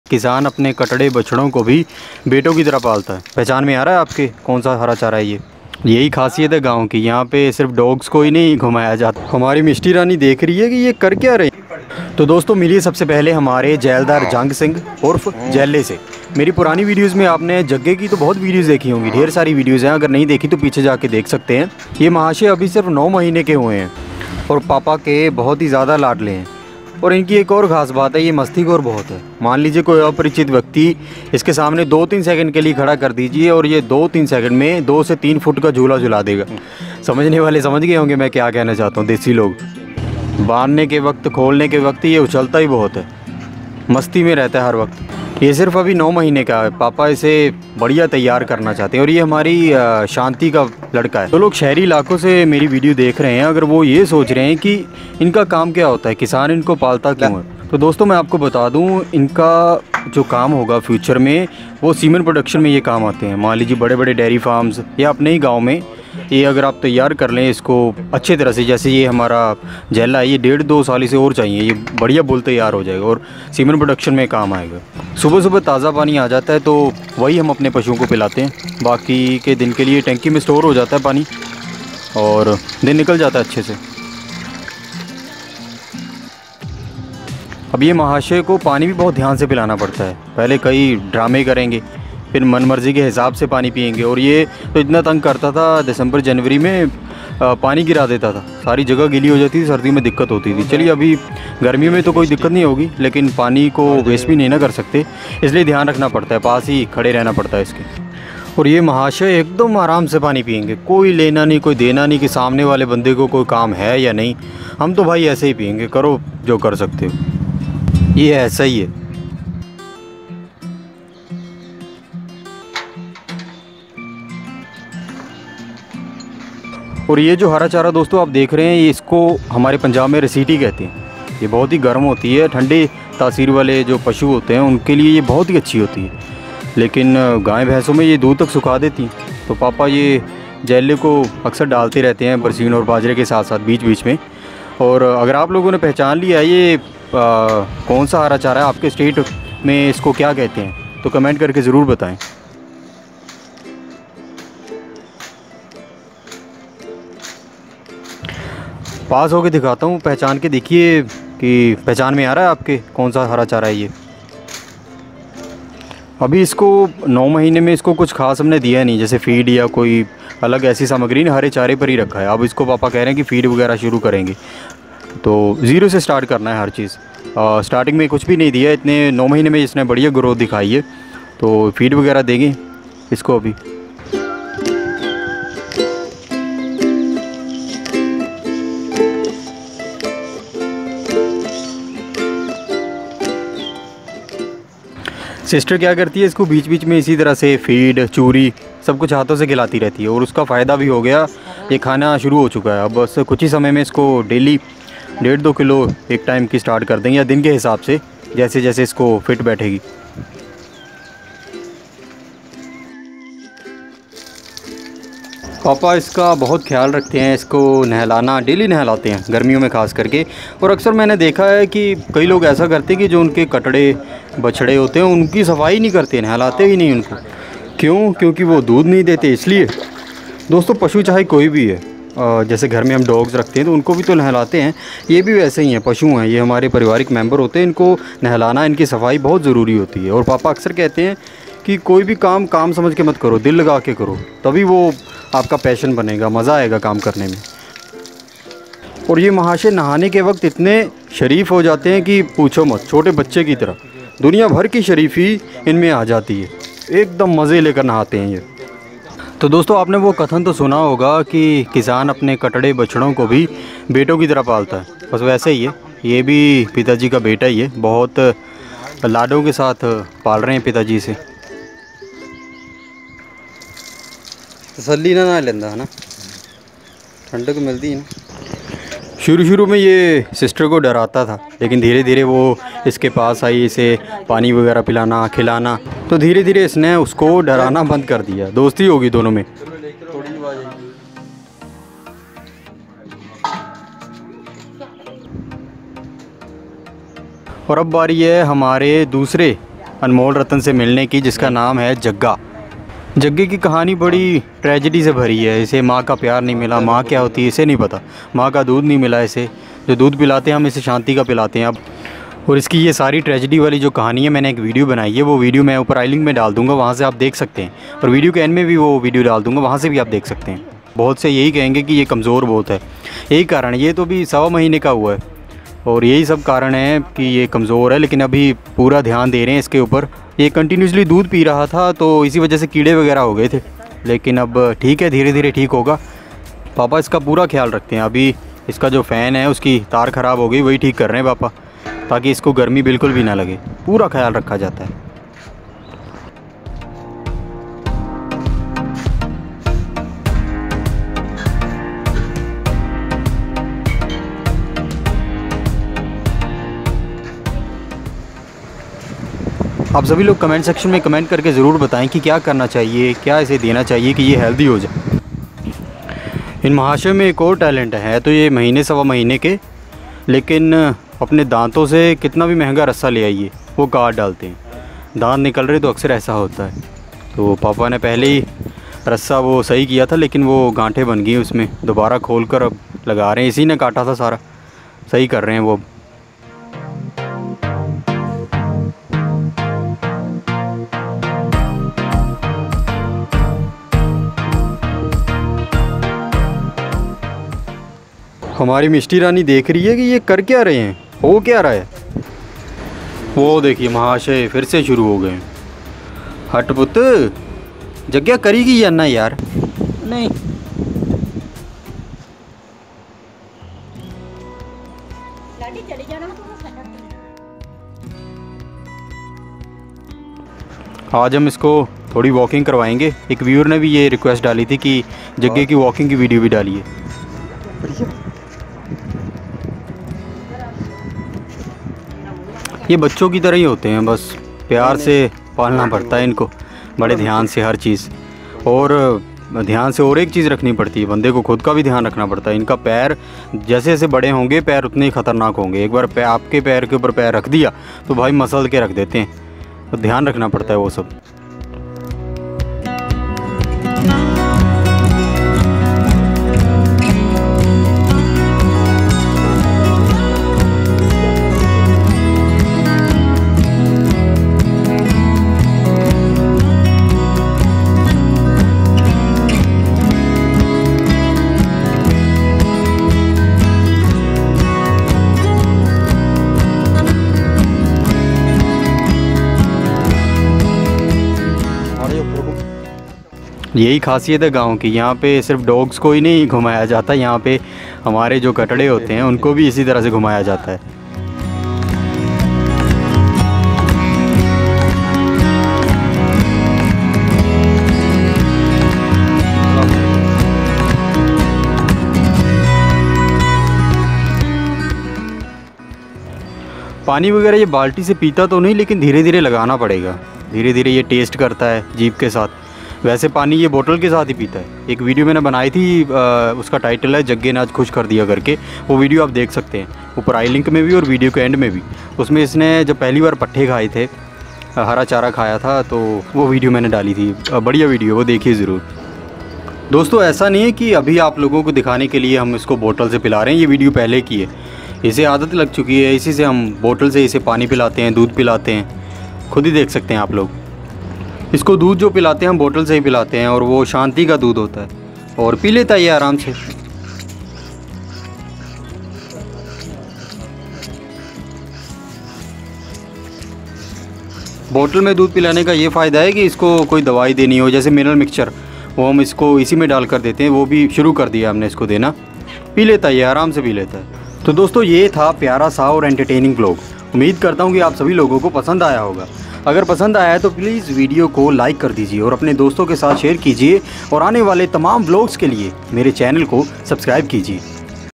Mere Do Anmol Ratan ❤|| Buffalo Baby Video || Buffalo Baby Drinking Milk
Aaj Ki Video Mei Aapki Mulakaat Hogi Hmaare Do Anmol Ratano Se .
Yaani Ki Jaile Aur Jaggey Se.
Aaj Ki Video Aap Logon Ko Bahut Pasand Aayegi. Isliye Video Aakhir Tak Dekhiyega.
आज की वीडियो में आपकी मुलाकात होगी हमारे दो अनमोल रतनों से।
यानी कि जैले और जग्गे से।
आज की वीडियो आप लोगों को बहुत पसंद आएगी, इसलिए वीडियो आखिर तक देखियेगा।